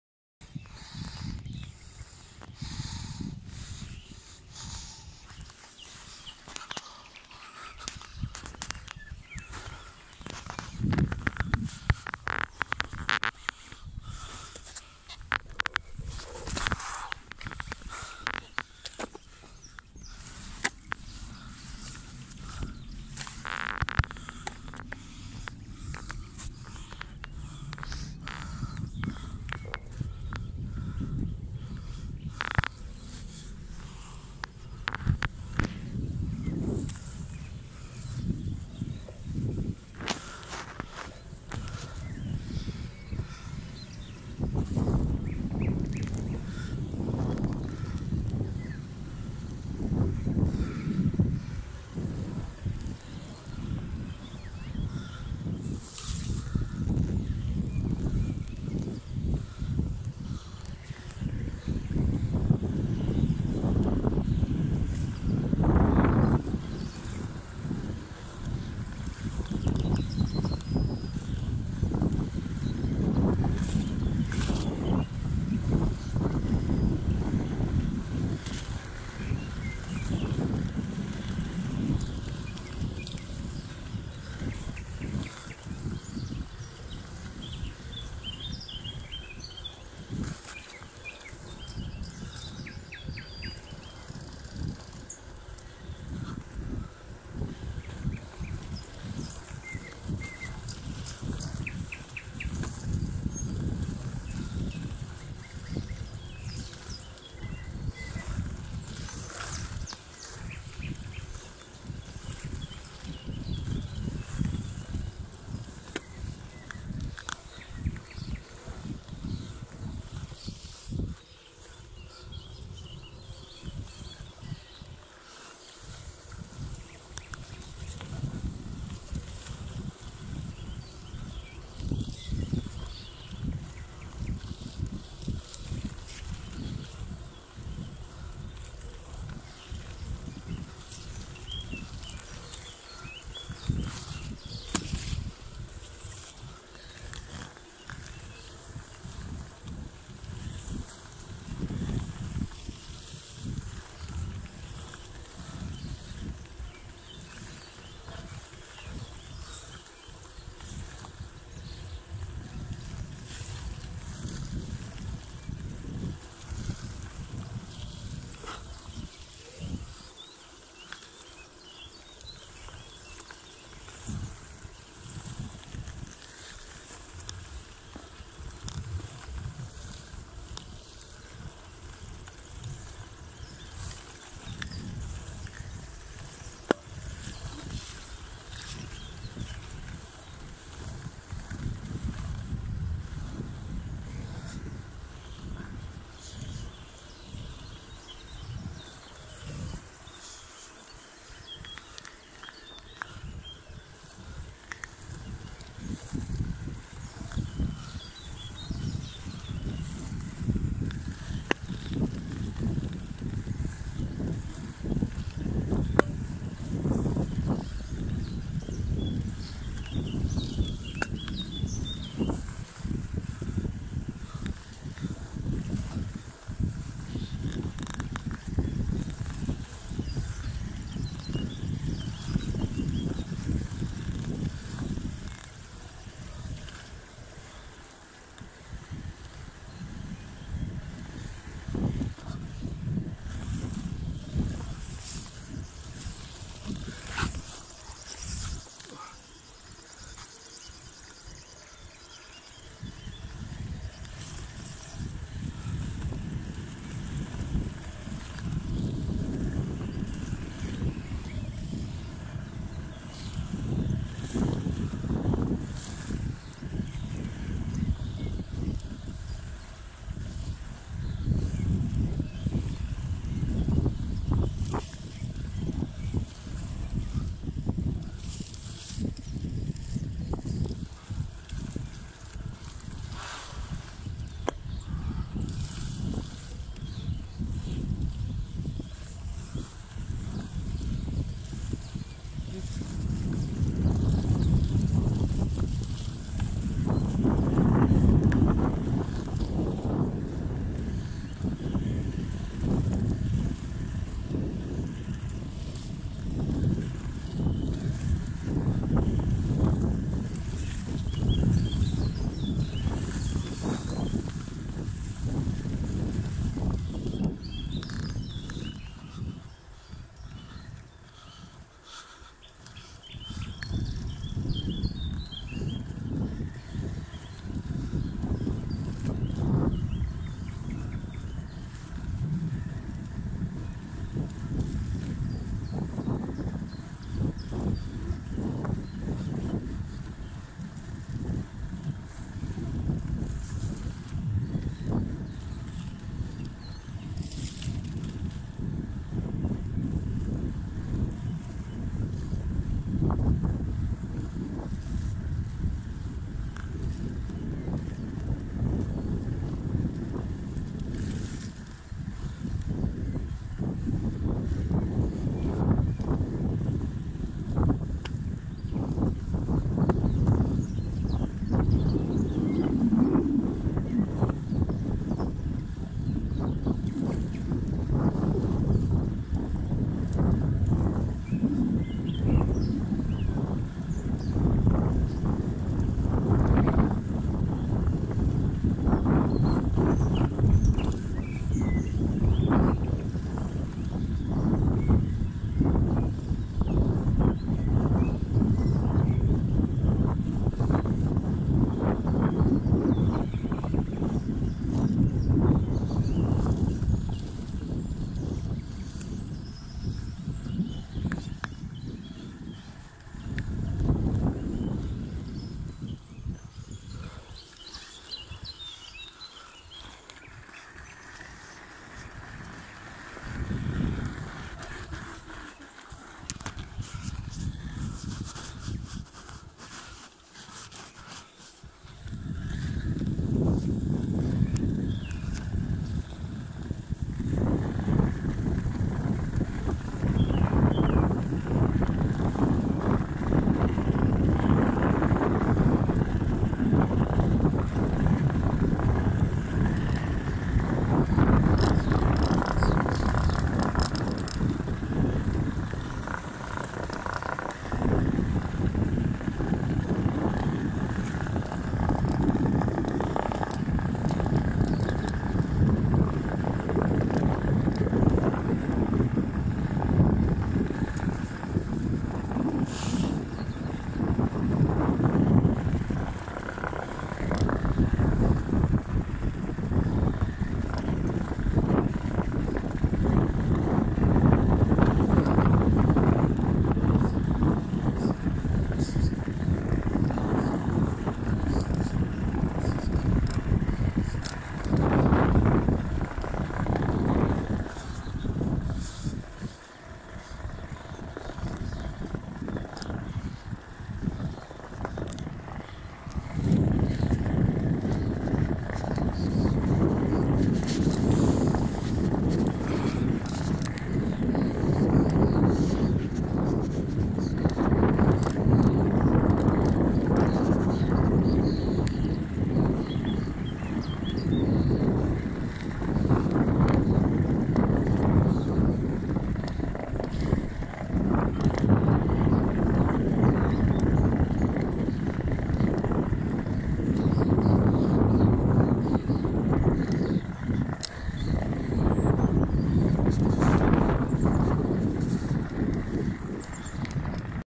31.5.2022 - Rzeka Narew o wczesnym poranku. Onewheel Ninja ➖®️➖ from Poland with Love. ΞLDORΔDO LΔBᔕ
Seria piękne poranki. Wał Narwii, okolice Nowy Dwor Mazowiecki. onewheel polska ELDORadoLabs -=Ξ ➖®️➖ Ξ=